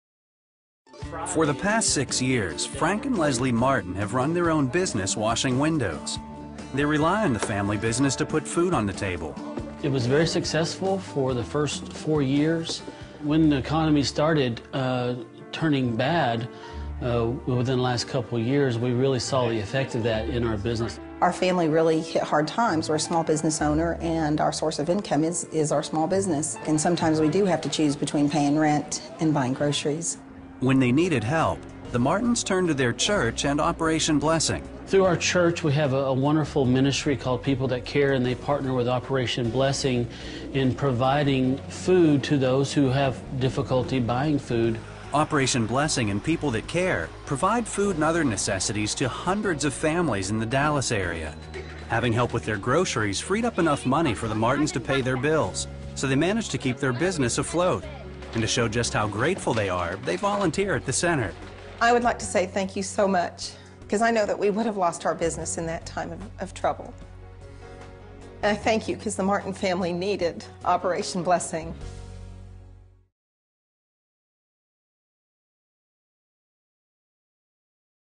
Pay Rent or Buy Food? - CBN.com
Barely keeping their business afloat and food on the table, Frank and Leslie Martin were desperate for help. Find out how you rescued their family and saved their business... The Christian Broadcasting Network CBN